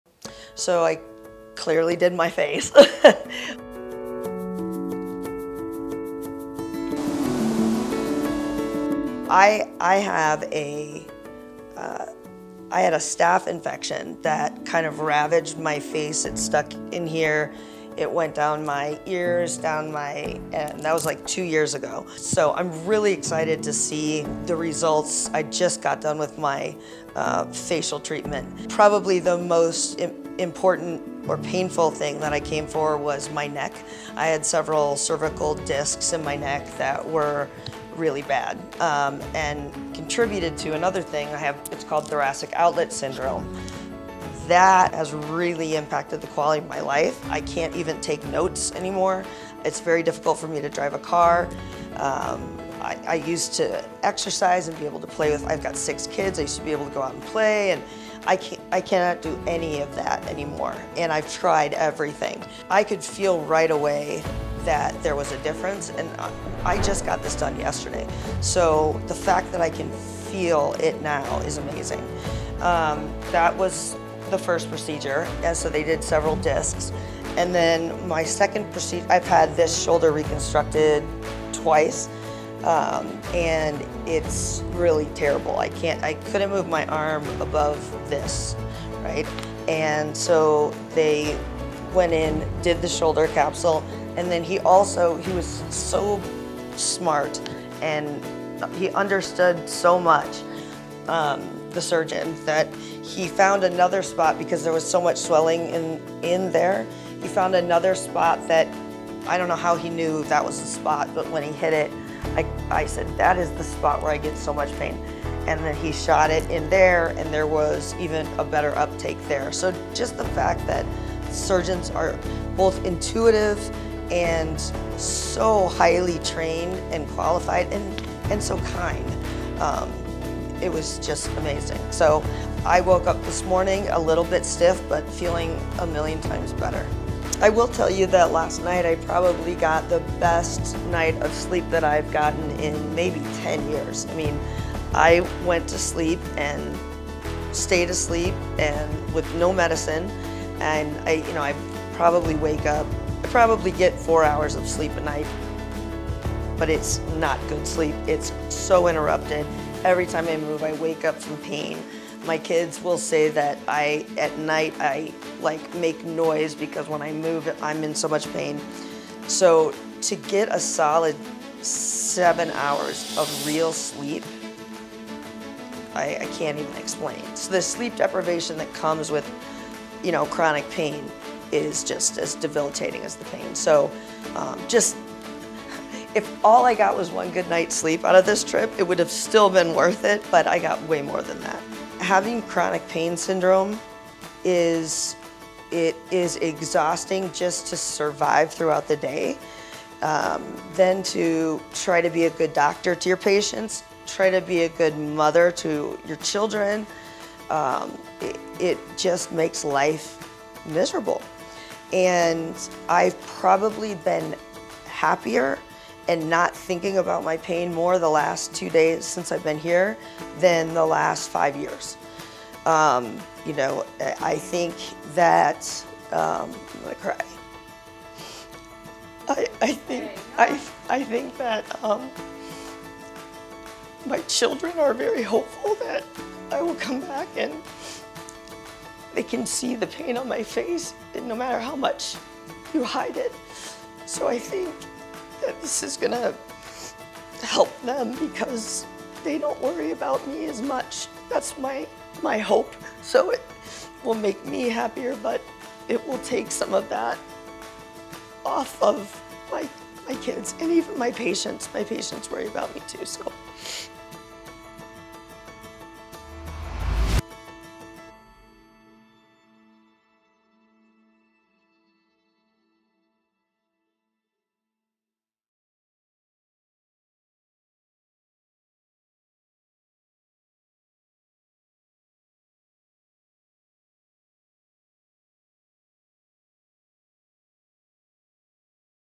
RIP pain : How Regenerative Medicine helped alleviate my chronic pain
Dr. Shannon suffers from chronic pain. It's been a part of her life for as long as she can remember.
Her doctors were at their wits end and no treatment really helped her until she found out about regenerative medicine. Regenerative medicine is the scientific term for stem cell therapy, and it was exactly what I needed to alleviate the pain that had become so much a part of my everyday life.